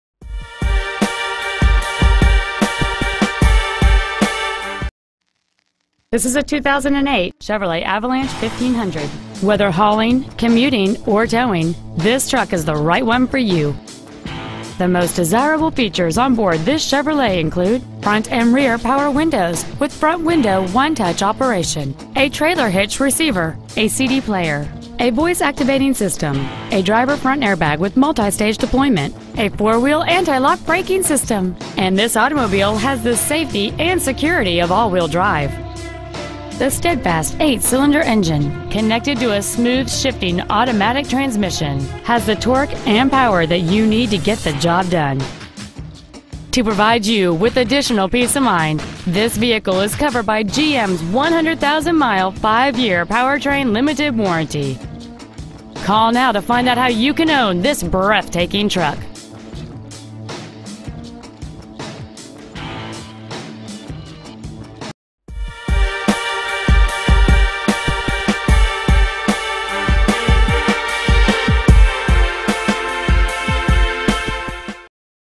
Pre-Owned 2008 Chevrolet Avalanche 1500 Appleton WI 54914
Bergstrom Chevrolet Cadillac Saab of Appleton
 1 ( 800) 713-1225

 Year: 2008
 Make: Chevrolet
 Model: Avalanche 1500
 Engine: 5.3L V-8cyl
 Trans.: Automatic
 Exterior: White
 Miles: 11,152